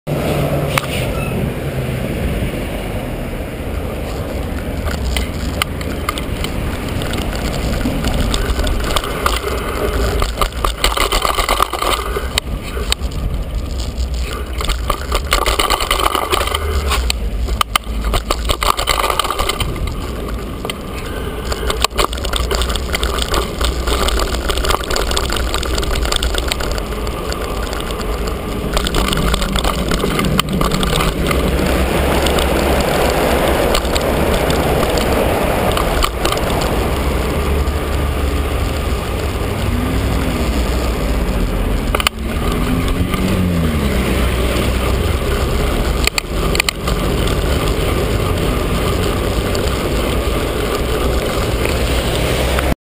GoPr - trepes
Ar velo pa trepēm, tā reāli GoPro uzvedas...
Ir ok!!!